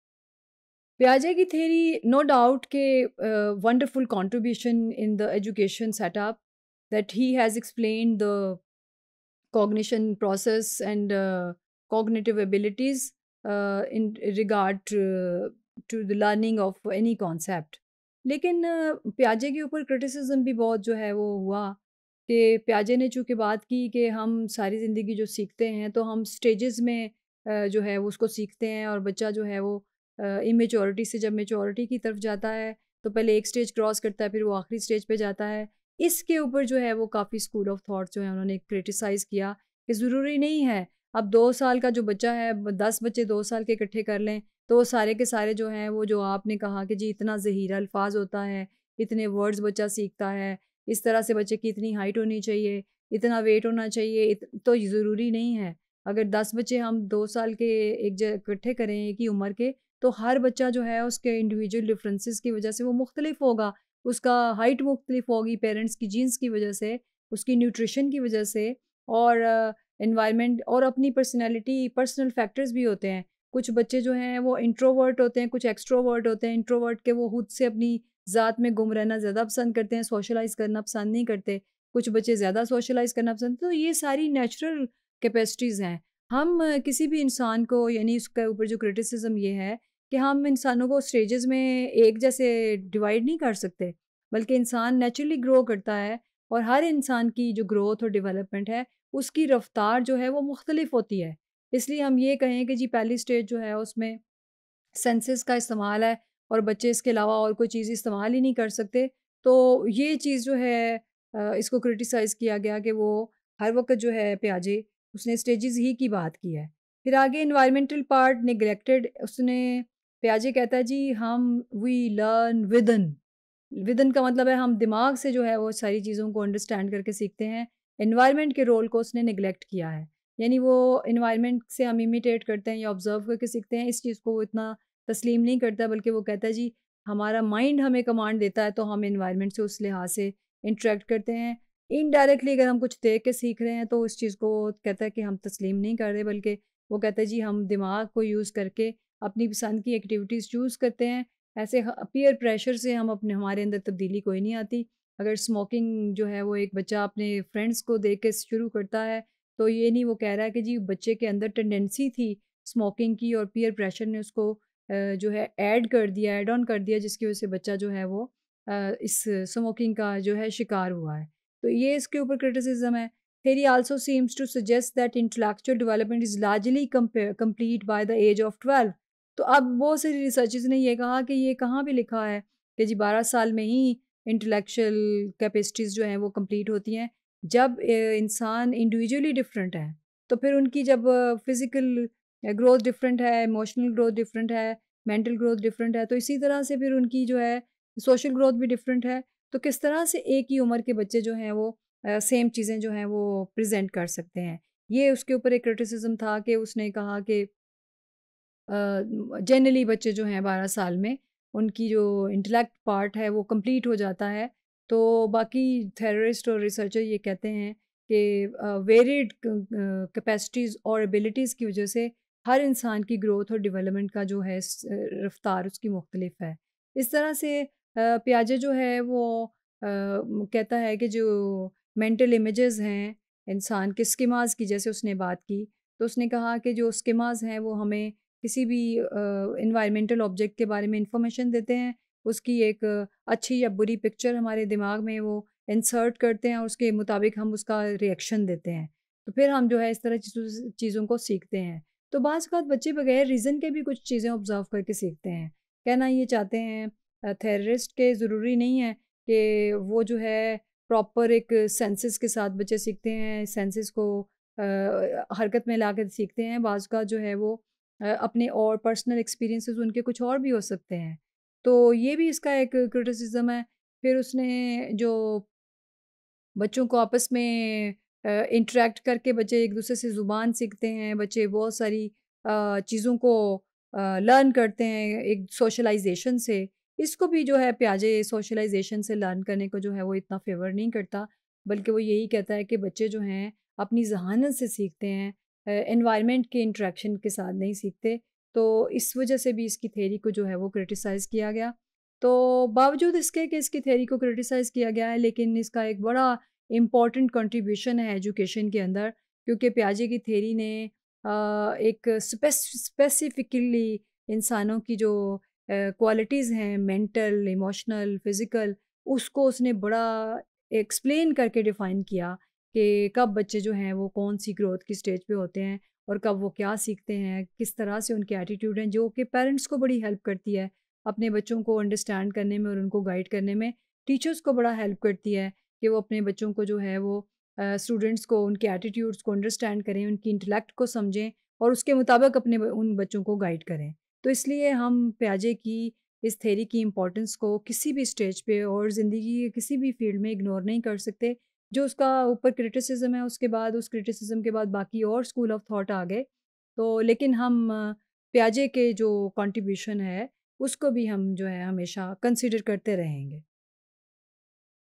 Criticism On Piaget's Theory | Human Development and Learning | EDU302_Topic060
Topic060 - Criticism On Piaget's Theory
By Dr. Sumaira Rashid